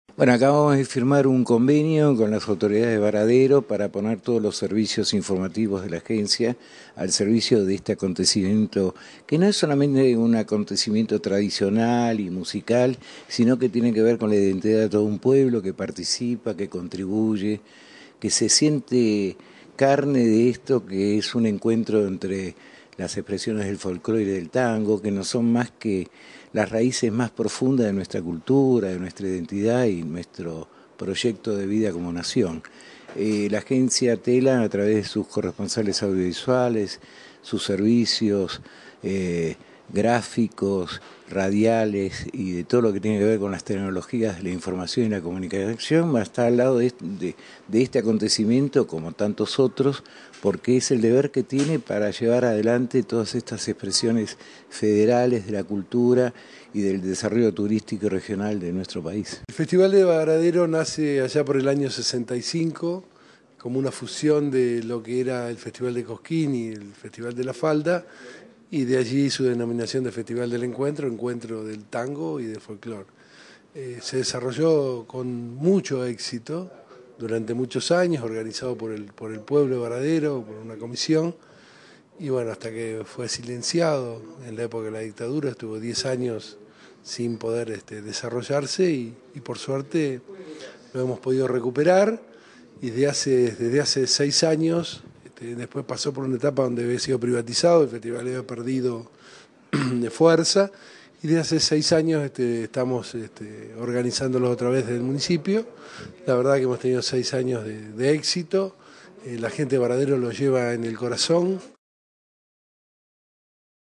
Télam firmó un convenio para difundir el Festival Musical de Baradero
La Agencia de Noticias Télam firmó un convenio con el partido de Baradero para difundir el festival musical de esa ciudad, que se realizará en el mes de febrero.
"El vínculo con Télam es muy importante porque se trata de un evento que realiza todo el pueblo de Baradero con mucho esfuerzo", dijo el intendente, Aldo Carossi.
El mandatario indicó que el festival -que reúne tango y folklore como géneros principales- tendrá lugar en el Anfiteatro Pedro Carossi del 9 al 12 de febrero, y contará con la presencia del Chaqueño Palavecino, Jorge Rojas, Los Nocheros y Abel Pintos, entre otros.
Carossi también destacó los atractivos turísticos que tiene la zona, como la reserva natural y los campings sobre el río Paraná que están dispuestos para los 8.000 visitantes.
Por su parte, el documentalista Roberto Vacca dijo que el convenio alcanzado "pone el potencial informativo de Télam (tanto en su versión gráfica, como audiovisual y radial) al servicio del festival" de Baradero.
El evento en la ciudad del noroeste bonaerense tiene lugar desde 1965 y, tras su prohibición por parte de la dictadura militar, se retomó con fuerza hace seis años, explicó el intendente.